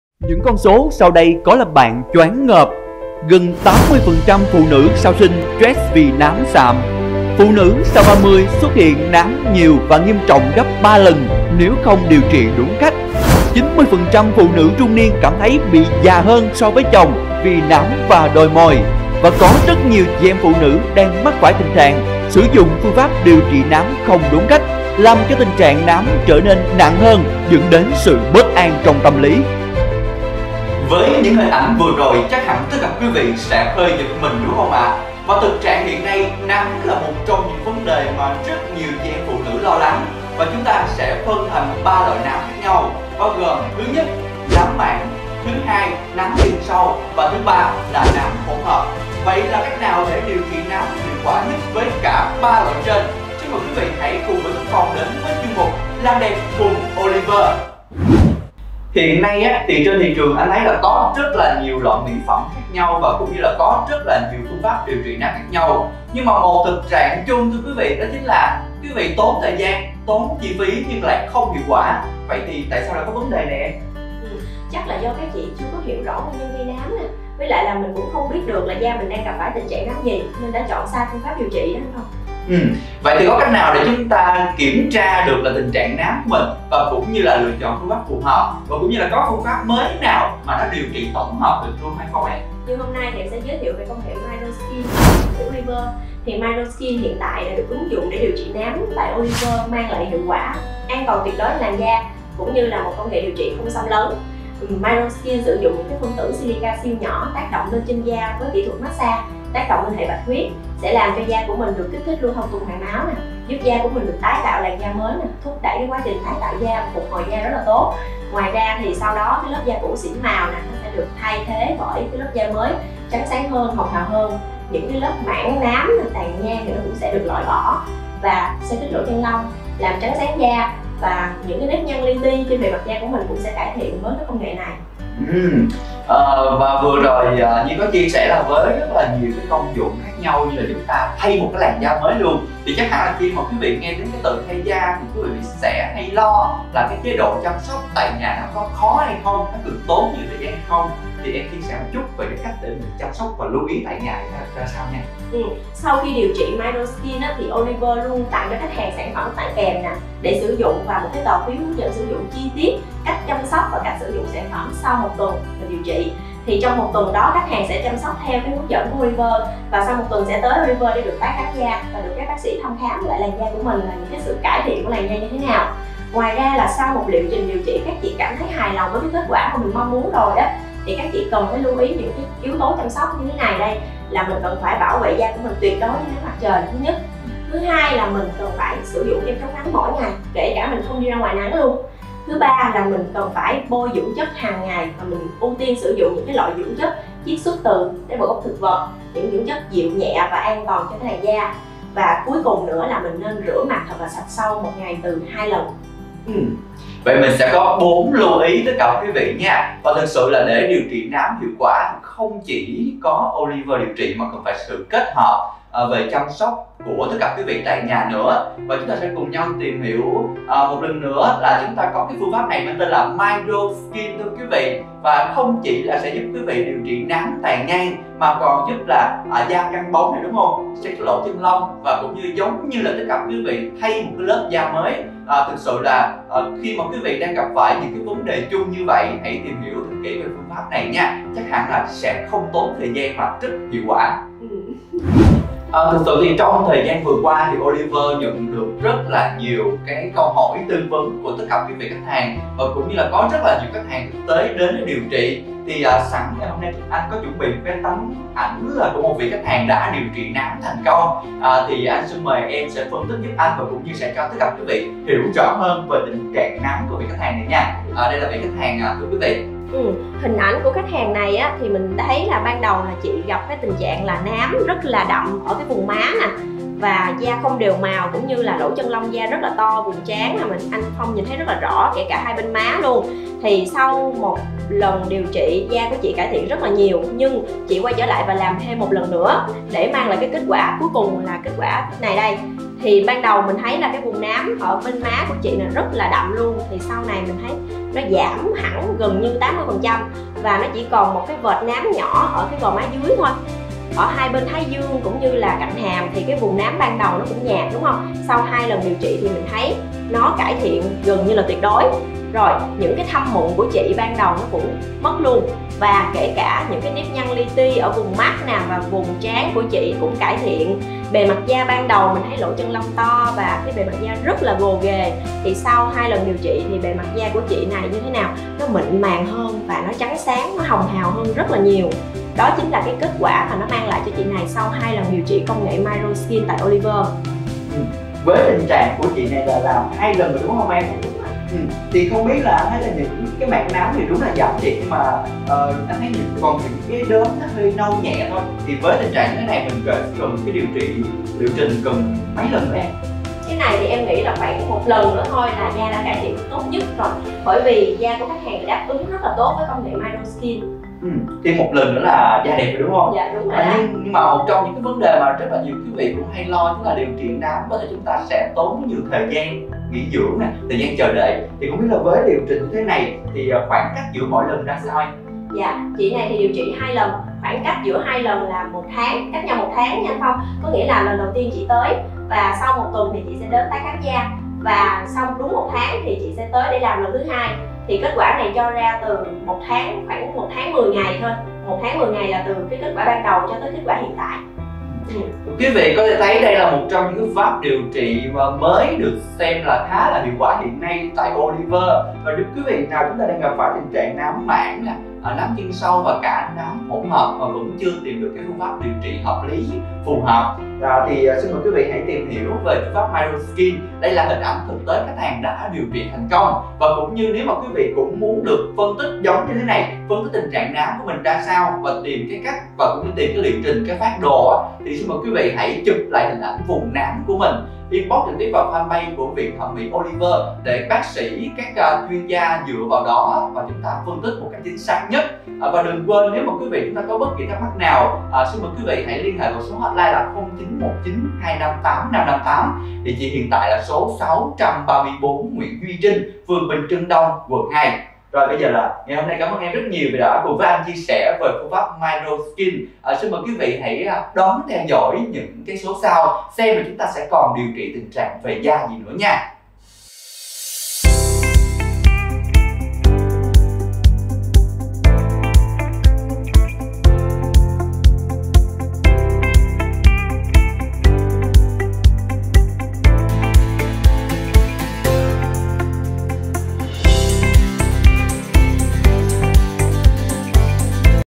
Chuyên Mục Làm Đẹp Cùng Oliver Tập 8 - MicroSkin là gì ?
🎀 Da sạm nám, tàn nhang được loại bỏ với công nghệ 𝗠𝗶𝗰𝗿𝗼𝗦𝗸𝗶𝗻:
☑️ Đẩy lùi nám, tàn nhang; trắng sáng bật tone giúp da đều màu.
☑️ Tăng sinh Collagen, da căng bóng theo từng buổi.
☑️ Se khít lỗ chân lông, mờ sẹo, vết thâm hiệu quả.
☑️ Cấp ẩm tối đa giúp da căng mướt, mịn màng.
☑️ Phục hồi làn da tổn thương nặng do ánh nắng mặt trời, sử dụng mỹ phẩm sai cách.
🏢 VIỆN THẨM MỸ OLIVER – Tôn vinh sắc đẹp Việt.
🏠 634 Nguyễn Duy Trinh, Bình Trưng Đông, Q.2.